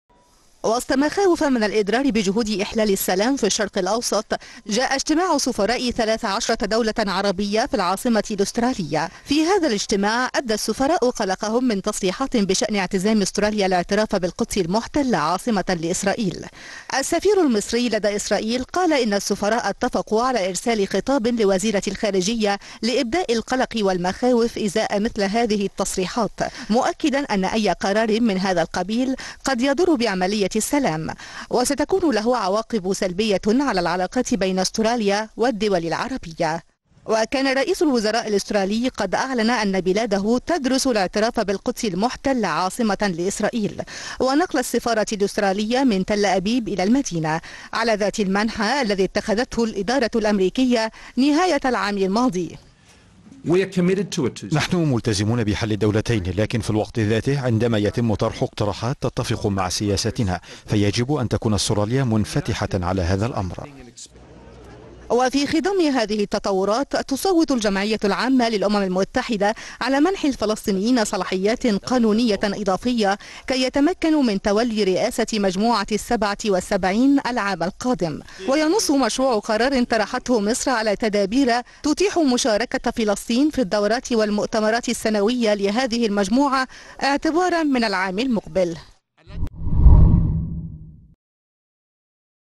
تقرير | 13 سفيرا عربيا في كانبرا يحذرون أستراليا من الاعتراف بالقدس عاصمة لإسرائيل
أعرب ثلاثة عشر سفيرا عربيا اجتمعوا في كانبرا عن قلقهم حيال سياسة أستراليا فيما يتعلق بخطوتها لنقل سفارتها بإسرائيل للقدس المحتلة.
يأتي ذلك فيما تصوت الجمعية العامة للأمم المتحدة بمنح الفلسطينيين صلاحيات قانونية إضافية، ليتمكنوا من تولي رئاسة مجموعة السبعة والسبعين في 2019، مزيد من التفاصيل في سياق التقرير التالي.